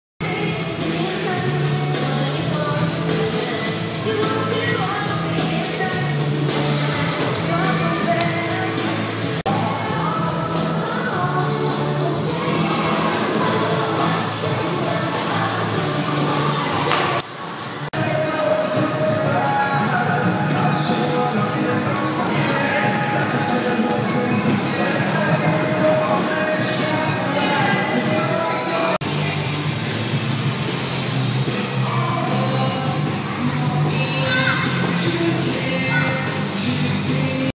SkyZone Cork Practice
Landed it 3 times..every time I landed one I had them record right after that. Never got it on camera. FML.